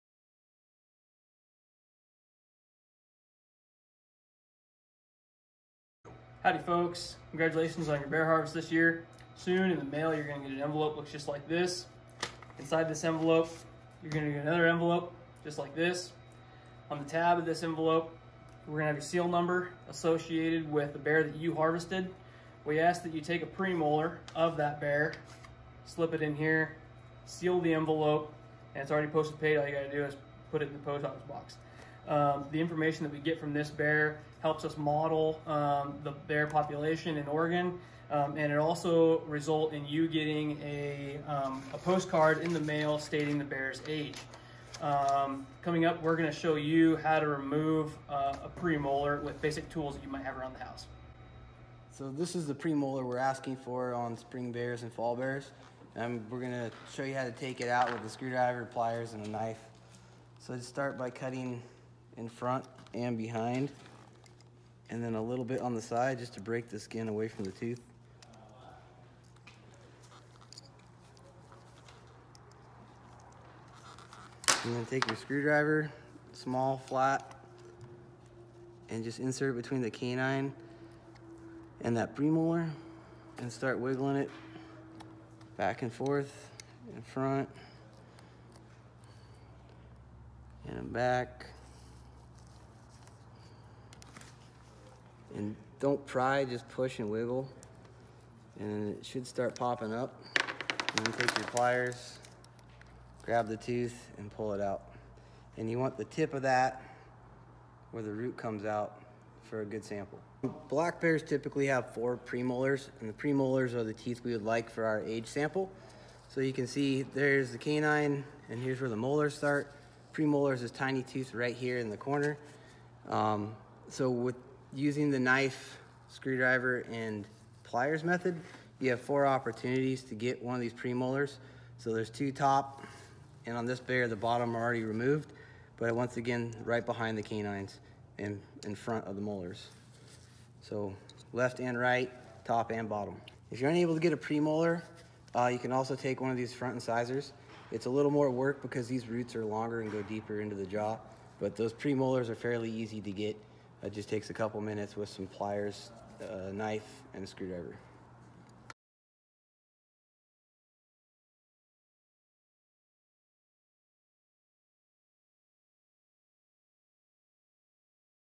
How to Collect and Submit a Black Bear Premolar Tooth
This video demonstrates how to collect a premolar tooth from a black bear using regular household tools. Wrapping the premolar tooth in some tissue and mailing it in an Oregon Dept of Fish & Wildlife postage-paid tooth envelope will provide valuable data used to model Oregon's black bear population. You will also receive a post card in the mail with information on the bear's estimated age.